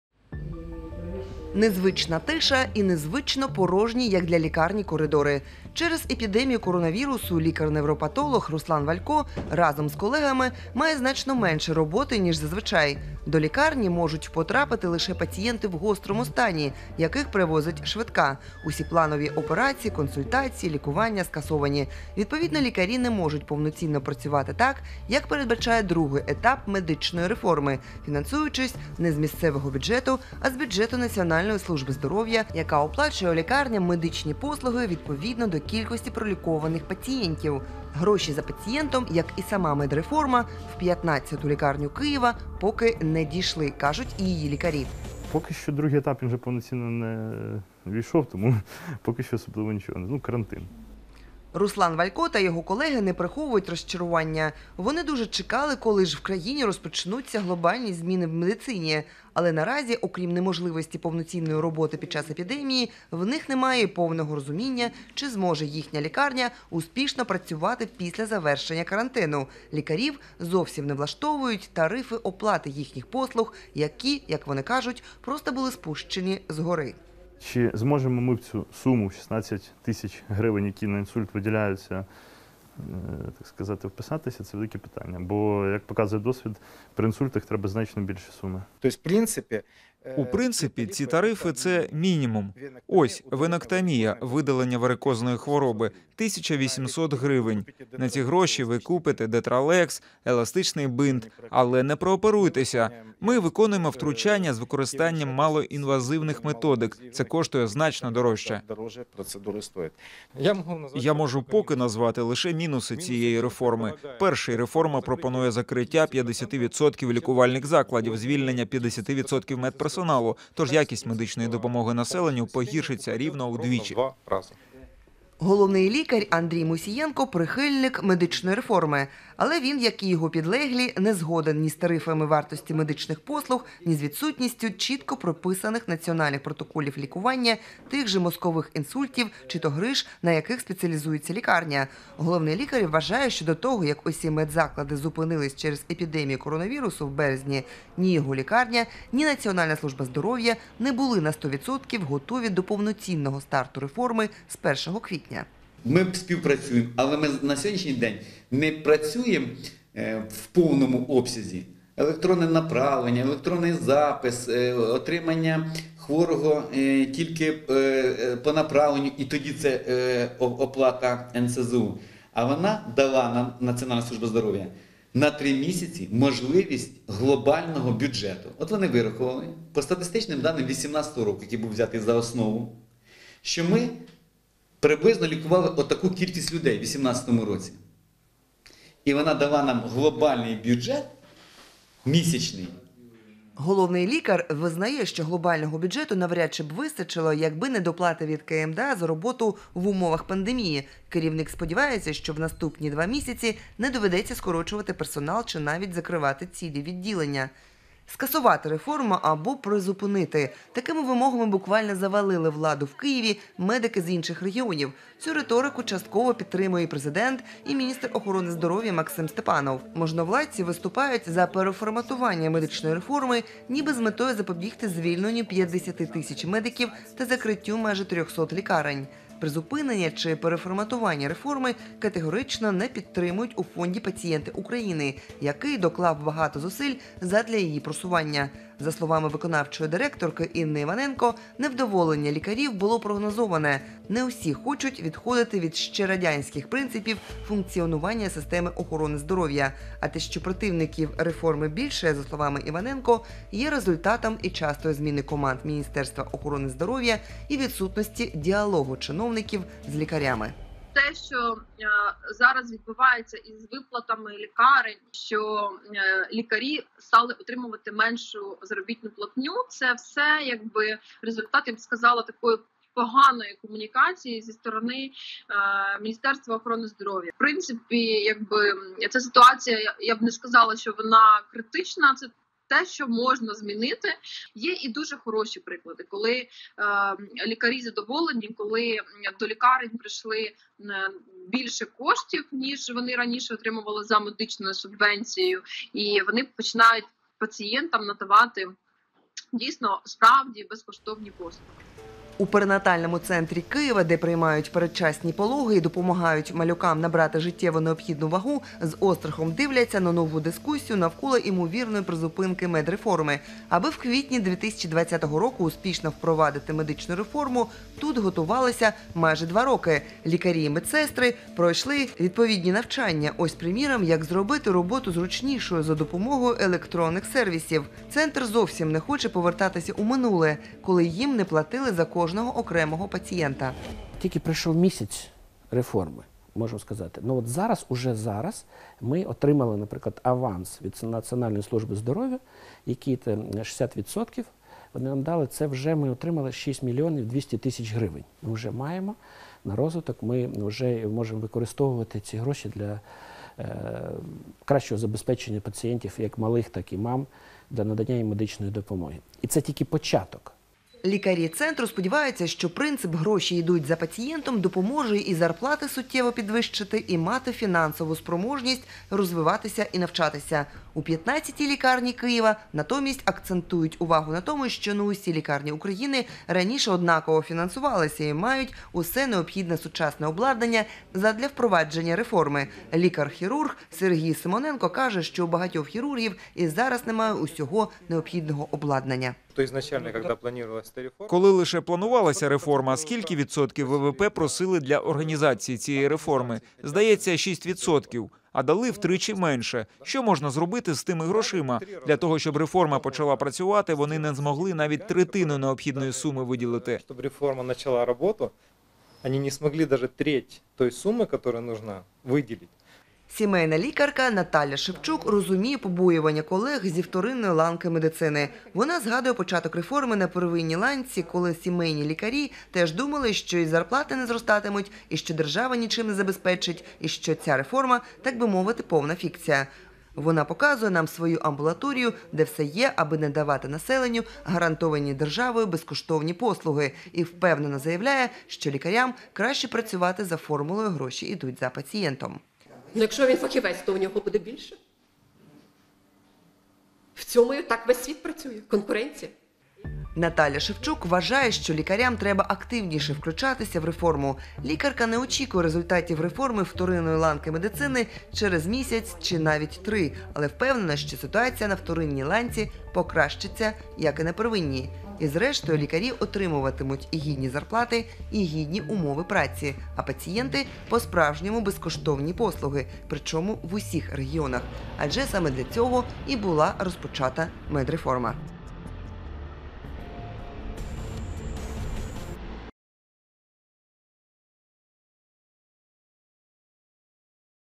Другий етап медреформи: коли гроші підуть за пацієнтом? | DW Ukrainian
З початком реформи вторинної медичної допомоги вузькі спеціалісти і профільні лікарні мали б отримувати кошти лише за фактично пролікованого пацієнта. Утім, наразі фінансуванням задоволені далеко не всі. DW з'ясувала, чому так і чи є позитивні приклади.

С началом реформы вторичной медицинской помощи узкие специалисты и профильные больницы должны были бы получать деньги только за фактически пролеченных пациентов. Впрочем, сейчас финансированием довольны далеко не все. DW выяснила, почему такая ситуация и есть ли позитивные примеры.